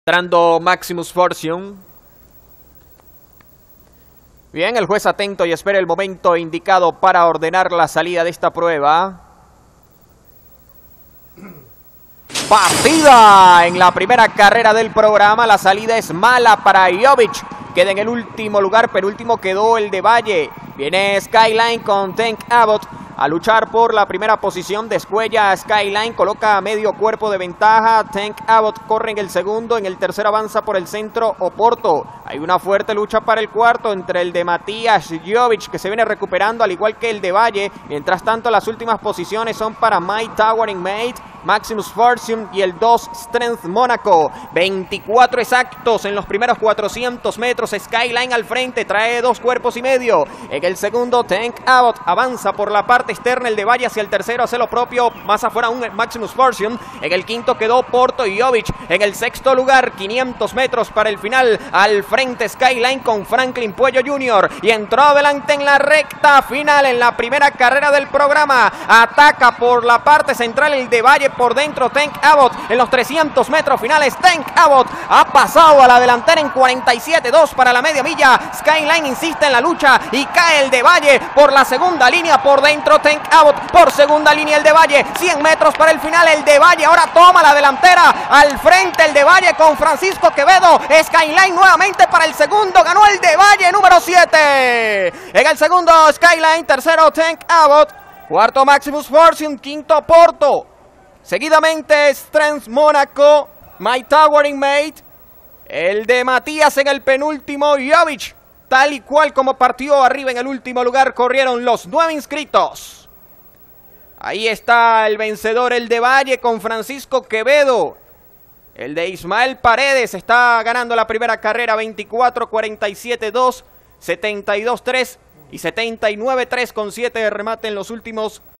C251| El de Valle | Hipódromo Internacional La Rinconada | Reunión 26 | 2024
"El de Valle" (7) triunfó en la 1era. carrera de la reunión 26 este domingo, con la monta del jinete profesional Francisco Quevedo y el entrenamiento de Ismael Paredes M.

👉 En el segundo lugar llegó "Skyline" (5).